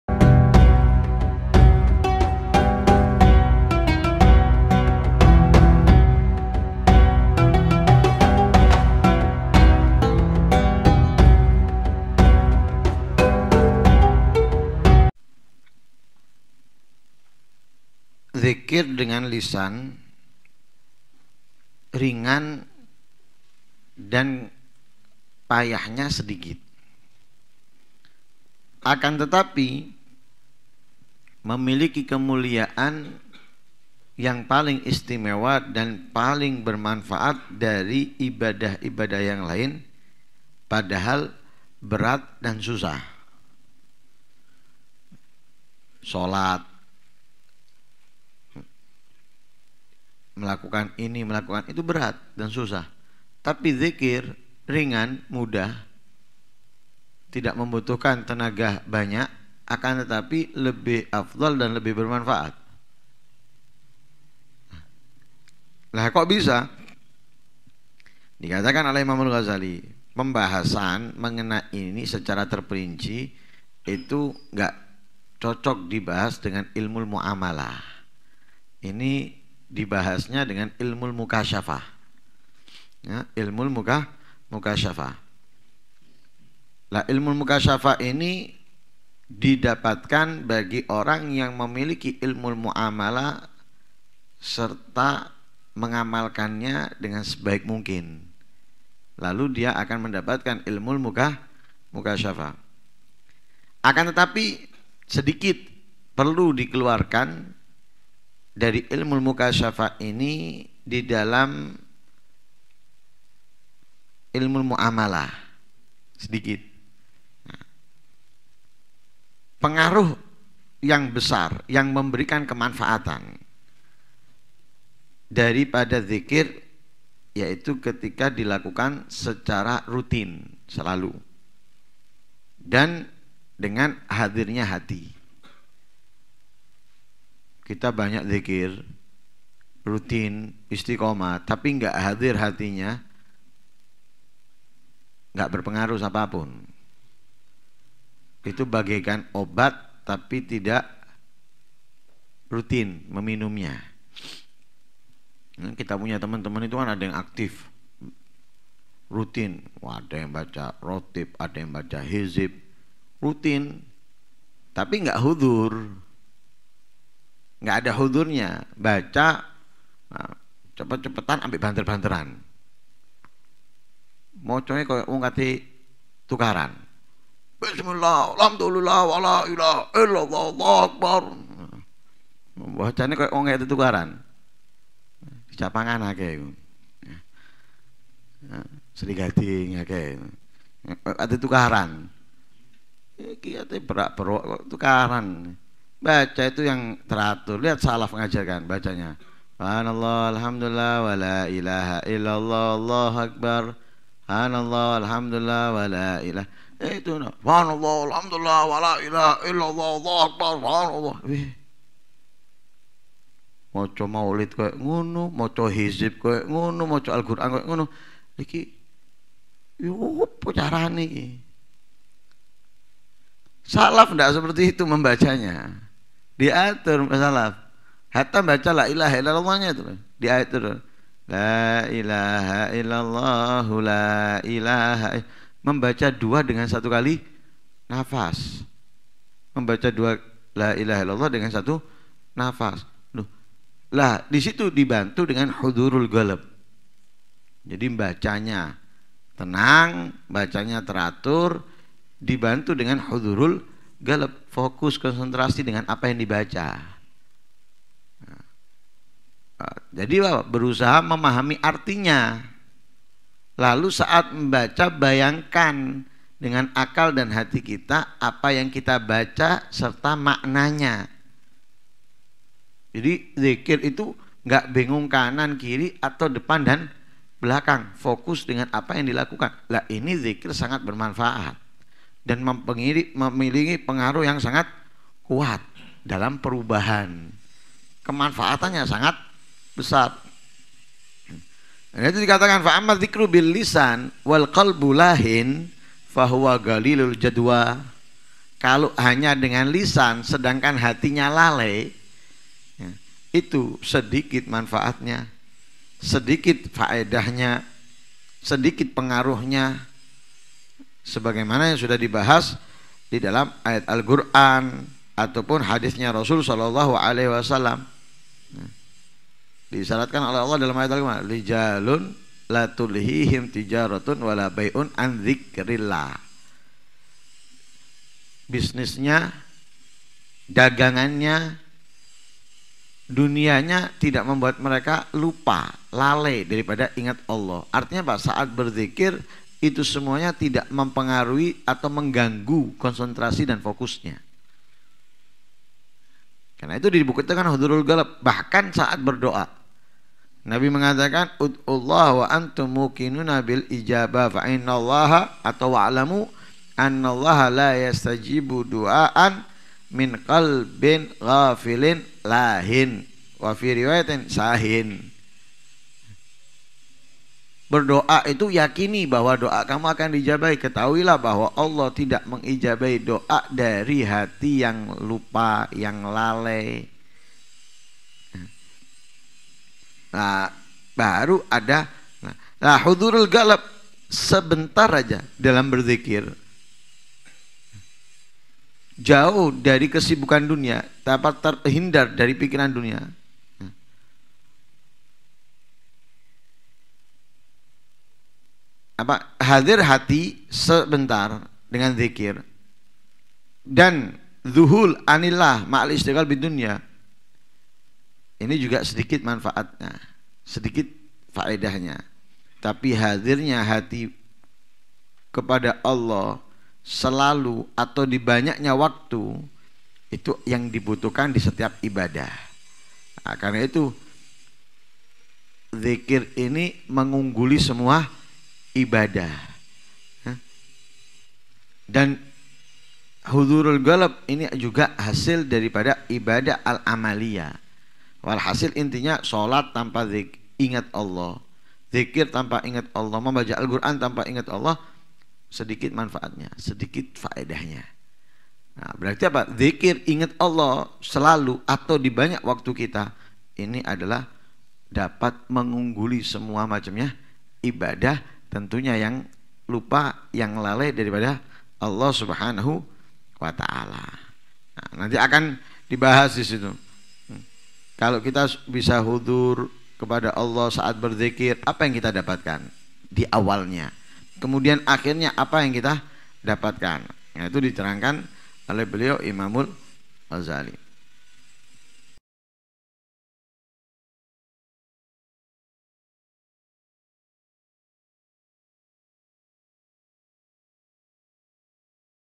Kenikmatan Berdzikir | Ihya' Ulumuddin (9 November 2020)
Majelis Rouha Pagi
Setiap Hari Senin,Selasa,Rabu & Sabtu
Pukul 07.00 s/d Selesai (UNTUK UMUM / MUSLIMIN)
Ma'had As Shofa' Kauman Gang 12 - Mojosari - Mojokerto
KITAB IHYA' ULUMUDDIN & FAWAIDUL MUKHTAROH